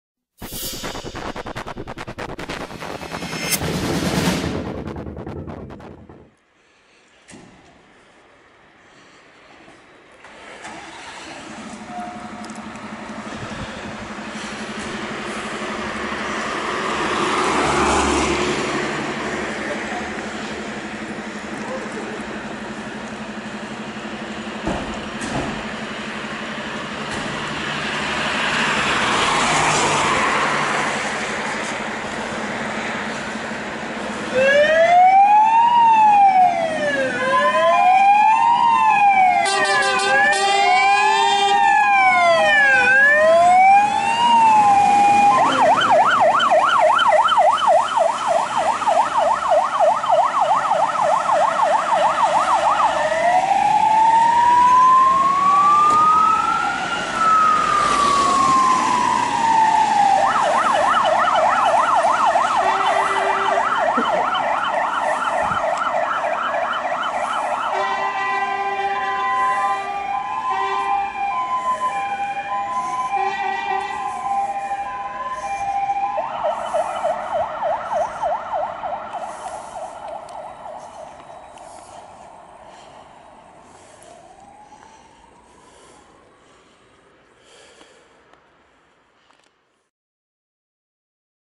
"GD 112": Wyjazd Alarmowy Sekcji 305[G]25 i 22 z JRG5 Gdańsk.
Kolejne zmiany w podziale i zamiast Volvo z JRG3 smiga małe Iveco.